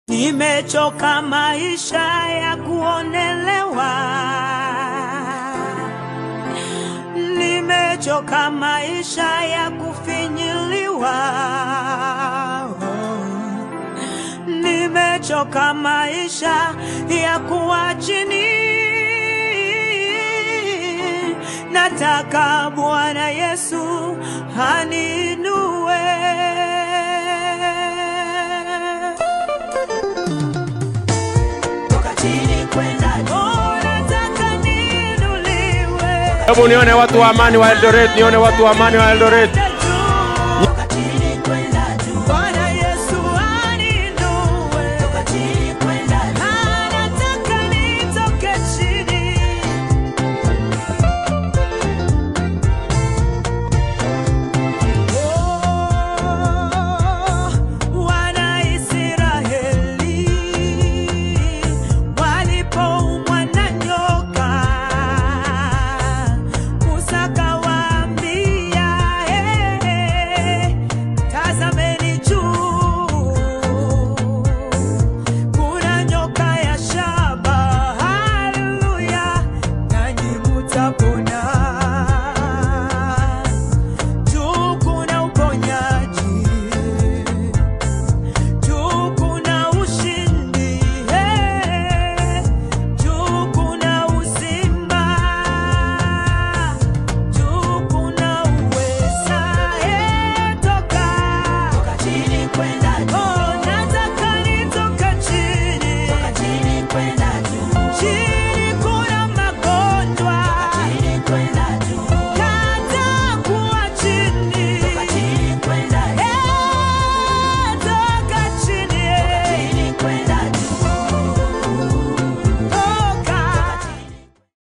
Ruto mega rally in Eldoret
Ruto mega rally in Eldoret song by Angela Chibalonza